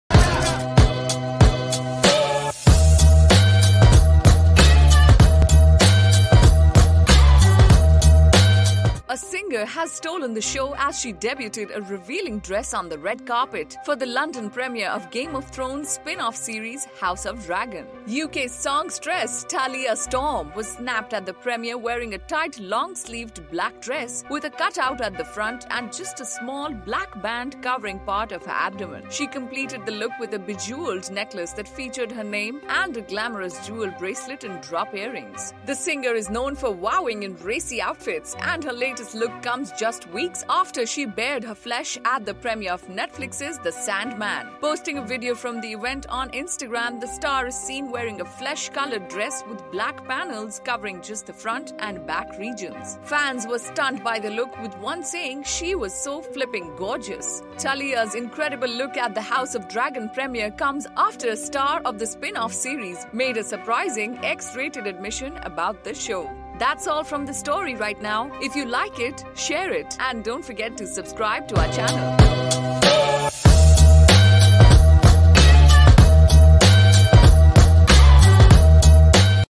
Tallia Storm steals show with racy look on House of Dragon red carpet
A singer has stolen the show as she debuted a revealing dress on the red carpet for the London premiere of Game of Thrones spin-off series House of Dragon.